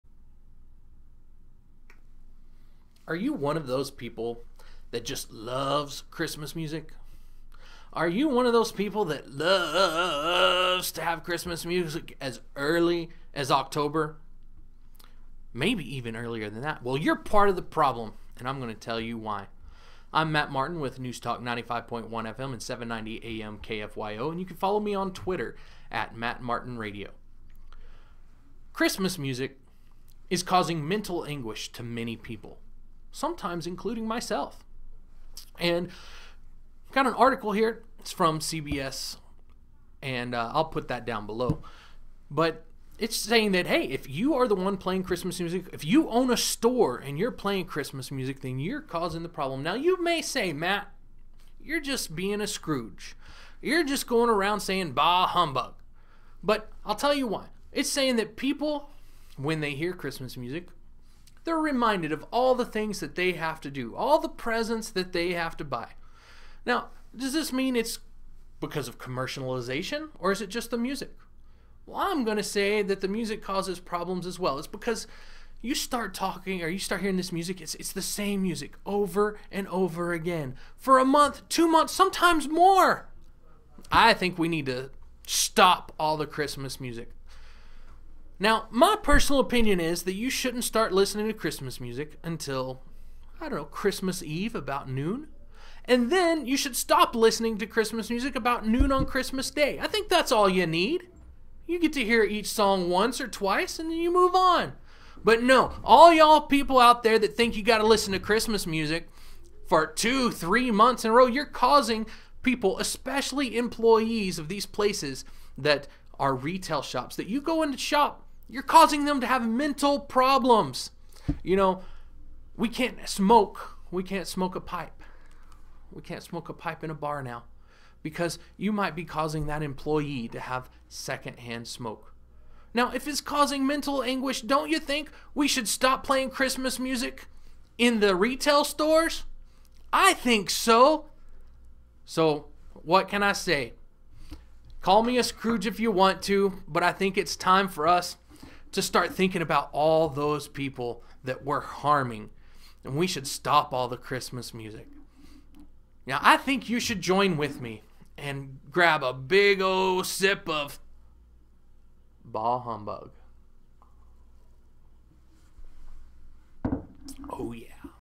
Christmas Music and Your Mental Health
Is Christmas music causing you mental anguish? Then you are not alone.  I am Matt Martin and you can catch me on 95.1fm, 790am kfyo from 6:00am to 8:30am, or you can always get it at kfyo.com.  I am tired of Christmas music being played so early, it is time for a change.  Maybe we should read the article below and think about someone other than ourselves...mainly, me.